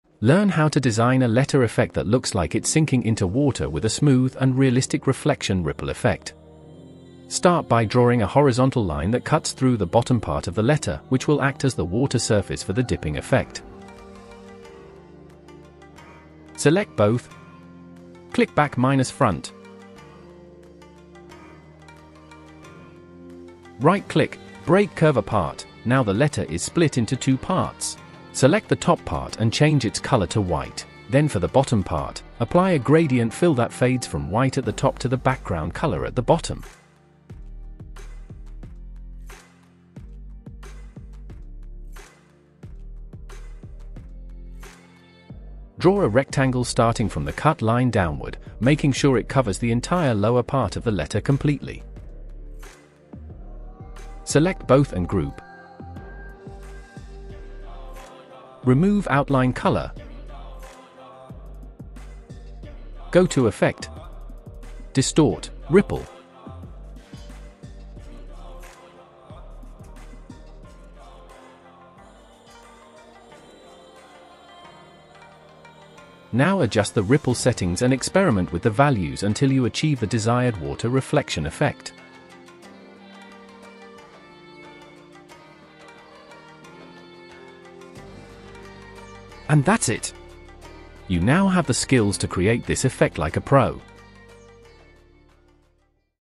Easy Graphic Design Trick You Can Try Today!
You’ll get the latest version and it supports the channel.
CorelDRAW Hacks You NEED To Know
Follow along with this CorelDRAW tutorial to Learn how to design a letter effect that looks like it’s sinking into water with a smooth and realistic reflection ripple effect! This step by step guide is perfect for graphic design enthusiasts who want to grow their skills using vector art. Enjoy this graphic design tutorial and build your drawing skills. 🚀